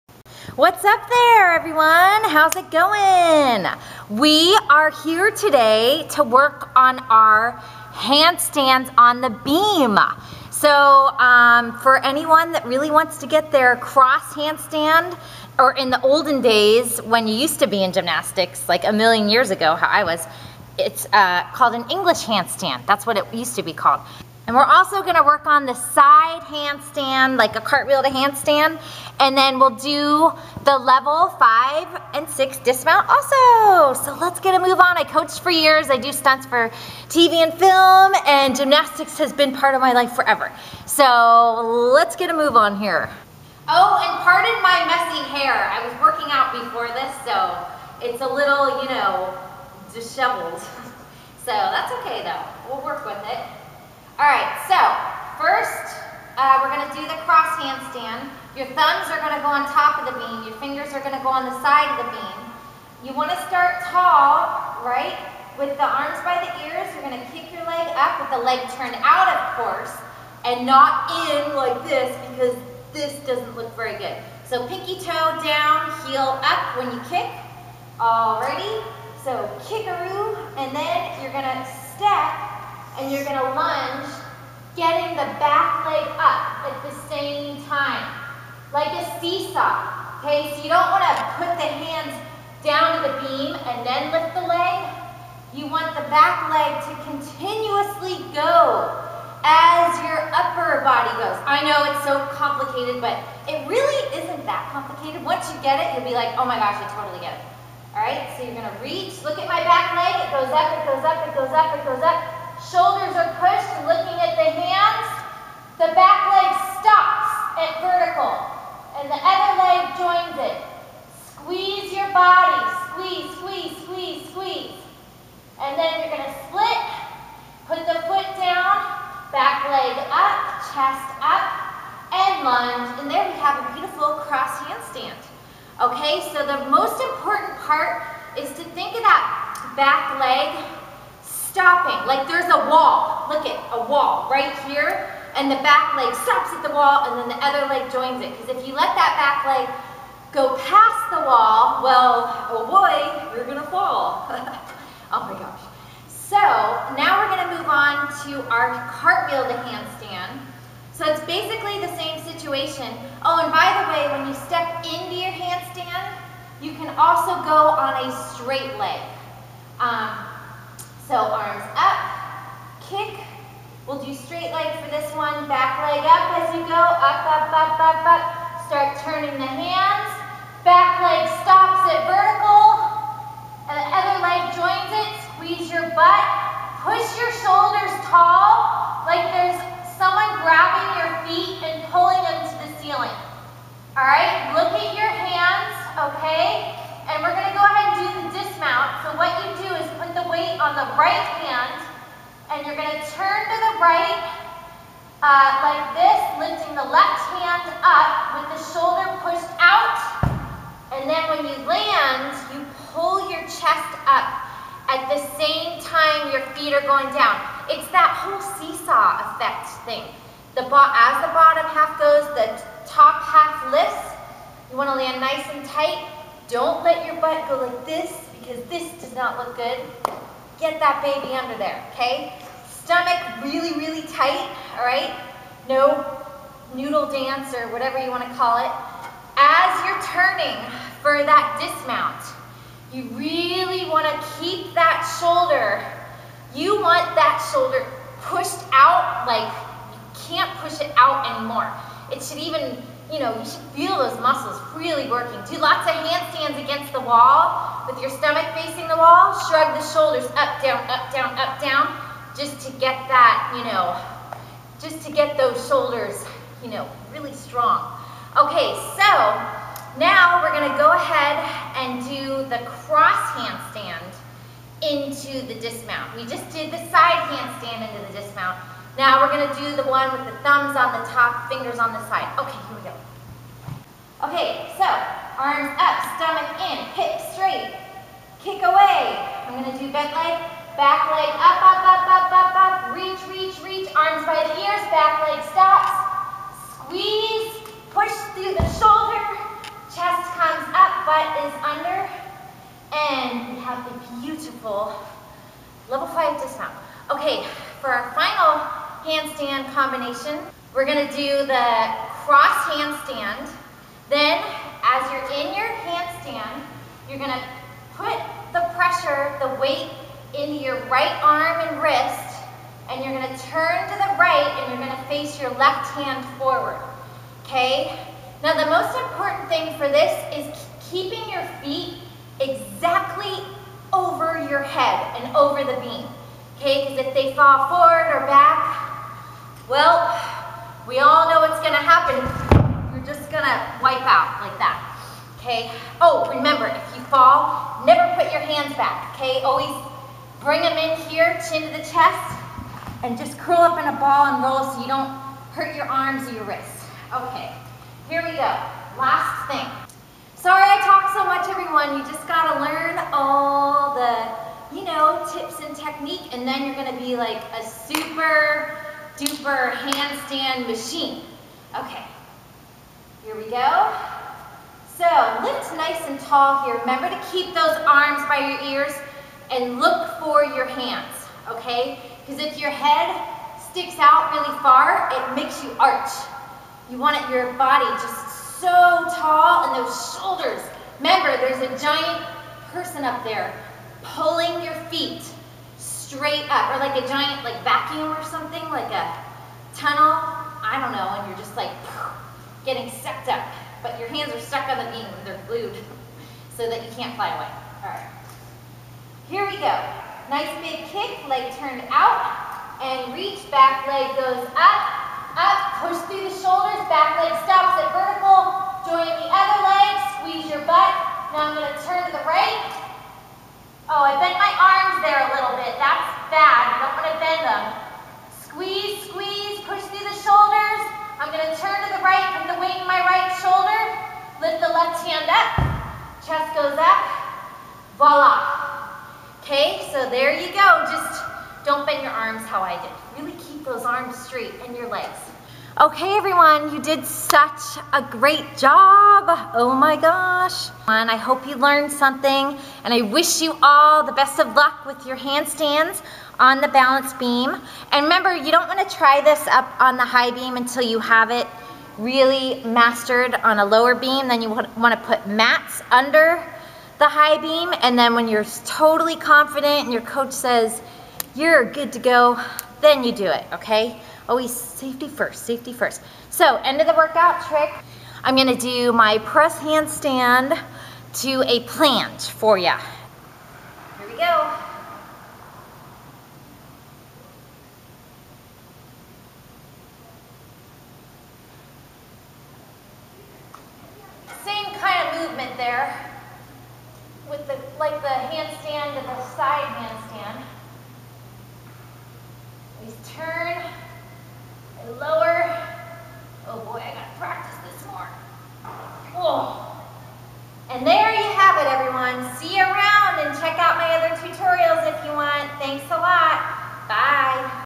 Handstands On The Balance Beam With Coach Meggin (Professional Gymnastics Coach)
Improve your handstands on beam and your level 5/6 dismoount too. Training with a stuntwoman, gymnast, and acrobat is fun!  Here's another tutorial you might like Facebook Fanpage Train at you own risk! I will not be responsible for any injuries.